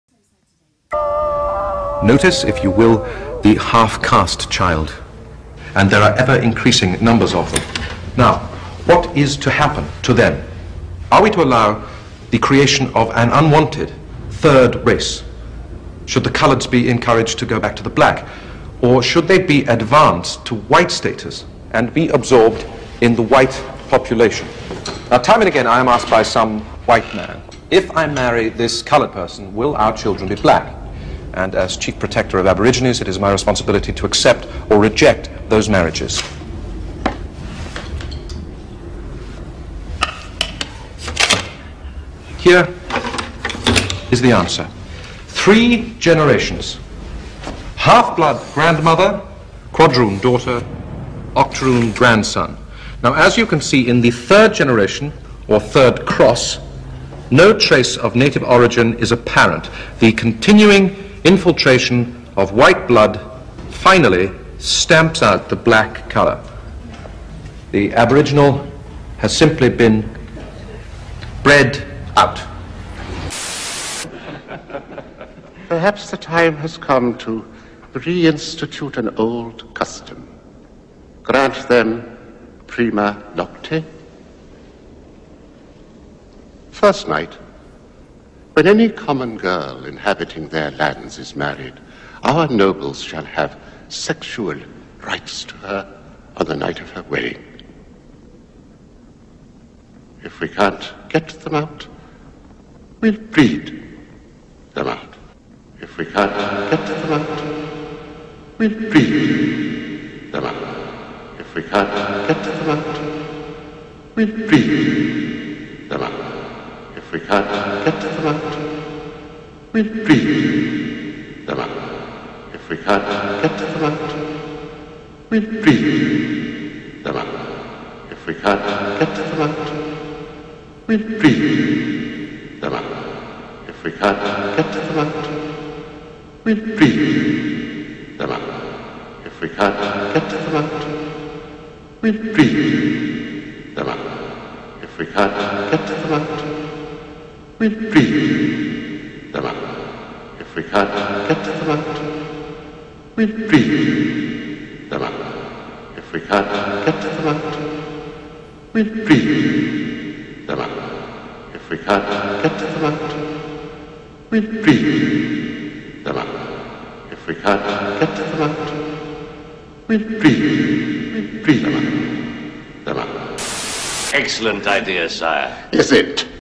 If we can’t get them out, We breed them out.
The genocide of black people through planned interbreeding.